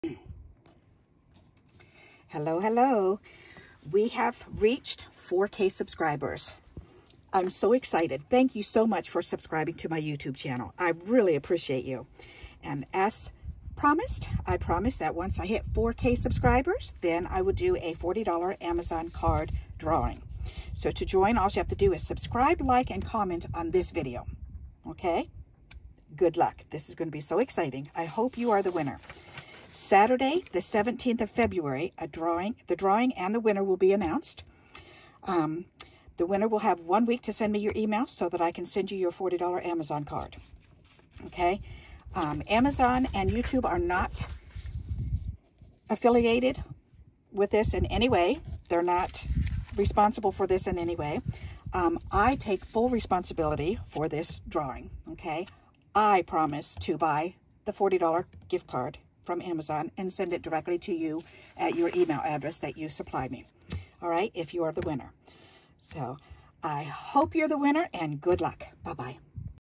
… CLOSED… 4k Subscriber GIVEAWAY AND DRAWING ($40. Amazon card). ……CLOSED……
2- LIKE
3- COMMENT on THIS VIDEO

Drawing will be HELD and WINNER will be announced: SATURDAY 17th February

Winner to be announced on YOUTUBE.
(and I will send you a message through your YouTube comment that you have won. )

Sincerely, Barbara

NOTE
YOUTUBE is not responsible in any way for this drawing.